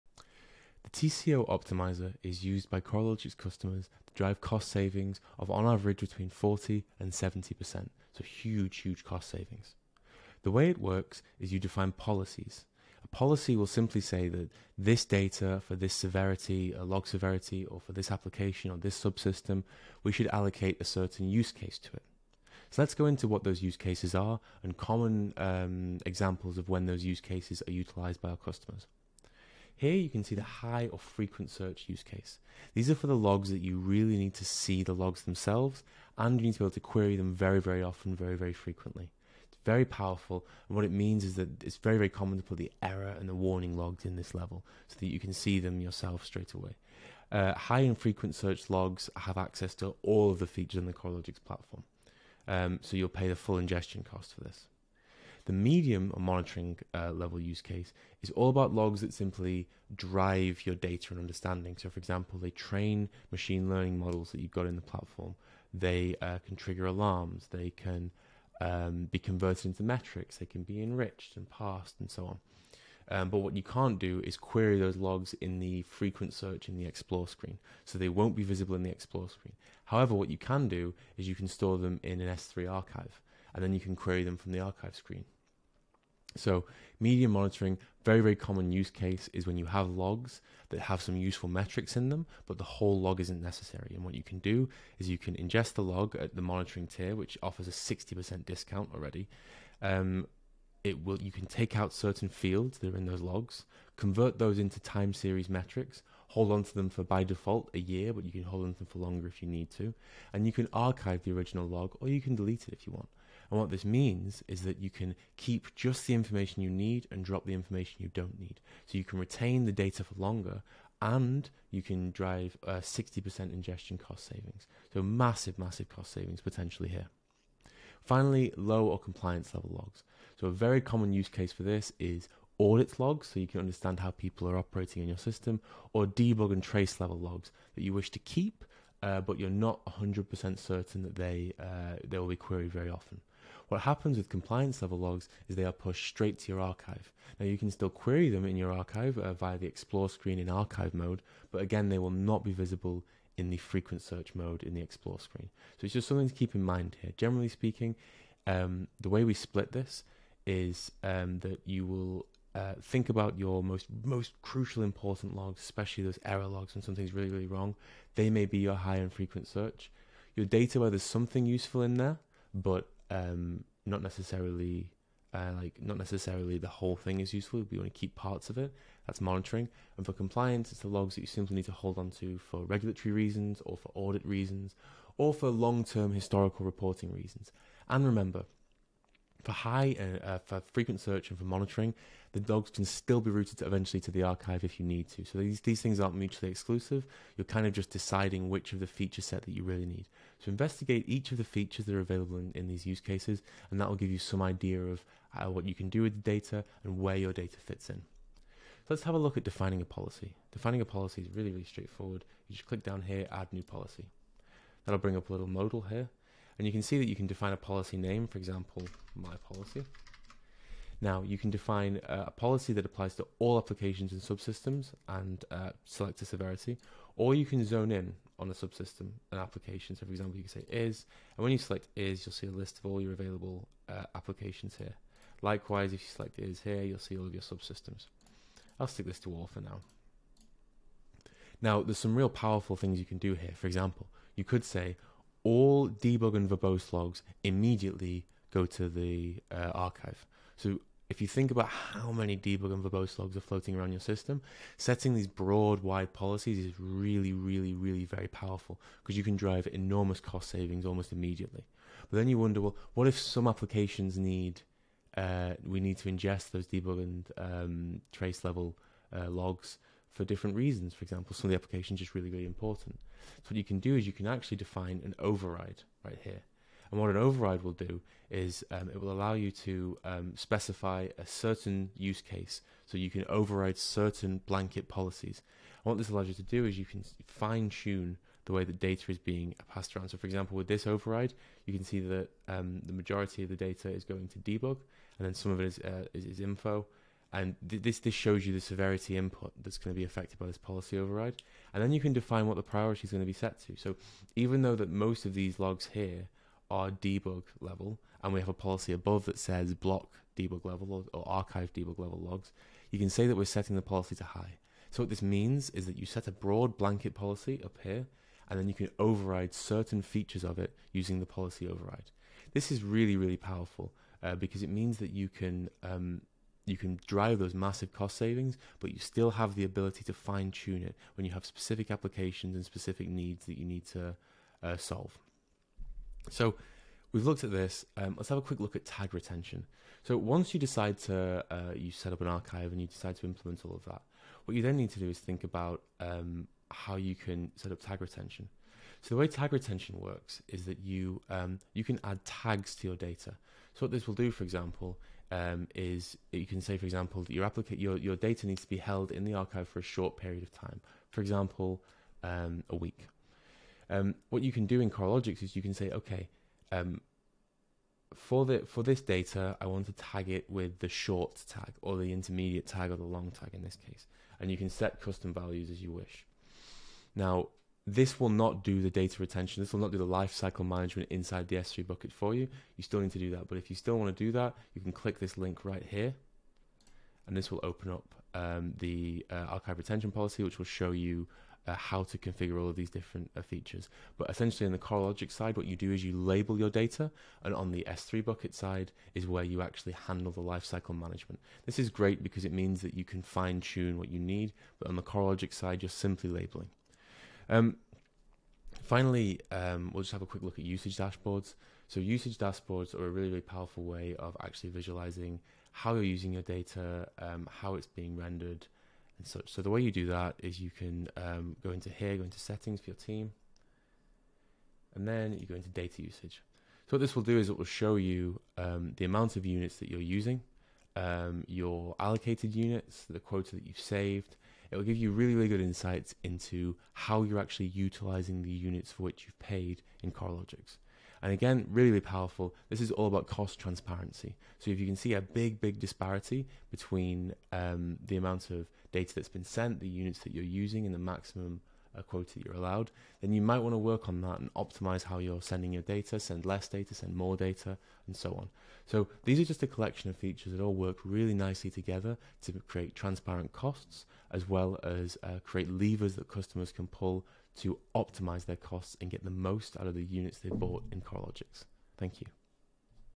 Coralogix Deep Dive - How to Save Between 40-70% with the TCO Optimizer
The TCO Optimizer is a key feature in the Coralogix cost optimization suite. Coralogix customers regularly see cost savings of between 40 and 70%, when compared to the prices quoted by the competition. With intelligent use of the TCO Optimizer, Coralogix even becomes more cost effective than a self-hosted ELK stack.

00:00 - Intro
00:30 - Breakdown of TCO Usecases
03:30 - Defining a Policy
04:20 - Policy Overrides
05:40 - Retention Tags in Coralogix
07:05 - Data Usage Dashboard